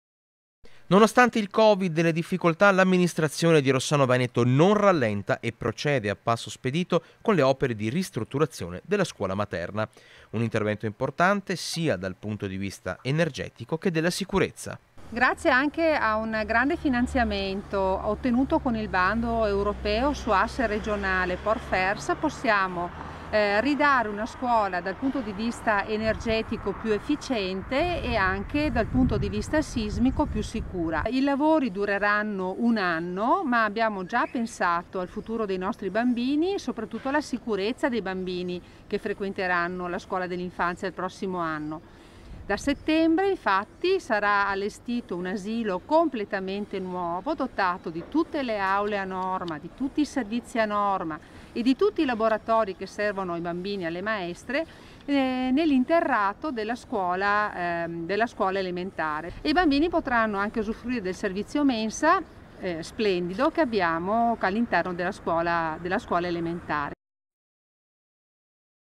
SCUOLA MATERNA PIÚ SICURA, I LAVORI DURERANNO UN ANNO | 21/03/2021
TG Bassano del Grappa 21/03/2021 - ROSSANO VENETO - A Rossano Veneto annunciati importanti lavori di ristrutturazione della Scuola Materna || Nonostante il Covid e le difficoltà, l'Amministrazione di Rossano Veneto non rallenta e procede a passo spedito con le opere di ristrutturazione della Scuola Materna. Un intervento importante per sia dal punto di vista energetico che della sicurezza. I lavori prenderanno il via a inizio luglio e dureranno un anno, durante il quali i bambini della Materna troveranno posto nella Scuola Elementare, con aule completamente nuove nell'arredamento e servizio mensa. - Intervistati MORENA MARTINI (Sindaco di Rossano Veneto) (Servizio di Alex Iuliano) - Segui Rete Veneta anche sul digitale terrestre! Visita il sito